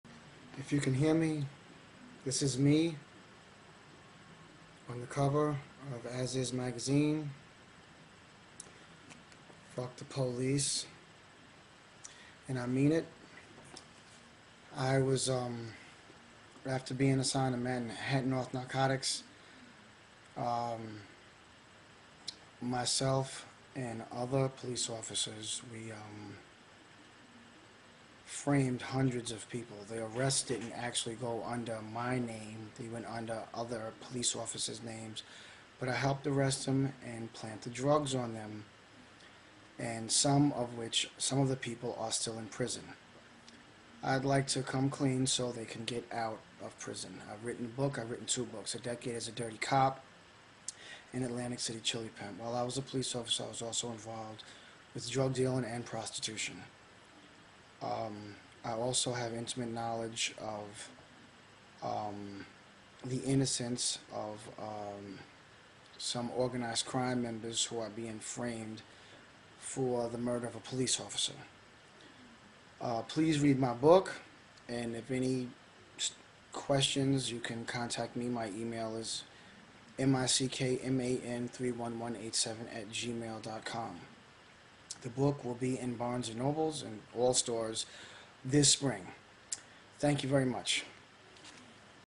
Former NYPD cop comes clean to set innocent people free.
Former NYPD cop speaks about mass corruption, drug dealing , set-ups, prostitution and other crimes that he and fellow cops committed while he was a NYPD cop. He wants his confessions to both exonerate innocent people still behind bars and warn the people of New York City what really goes on inside the NYPD.

 Let us support this ex-cop for having the courage to come forward with the truth that will set innocent people free. Let's not focus on this officers mistakes and lapse of judgment that he made when he was among thieves and criminals in the police department. Let's send other cops harboring the truth a message that it's never to late to do the right thing. This officer joins the ranks of Adil Polanco, Adrian Schoolcraft and Frank Serpico for speaking out against police corruption.

Listen to Ex-cop MICHAEL MICKMAN GOURDINE on Blog Radio